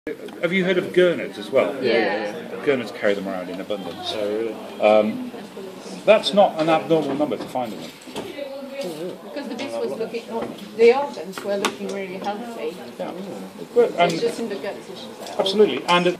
Parasites found within Lesser Spotted Dogfish (dissection)
During a dissection we found hundreds of parasitic worms within the insides of a dogfish.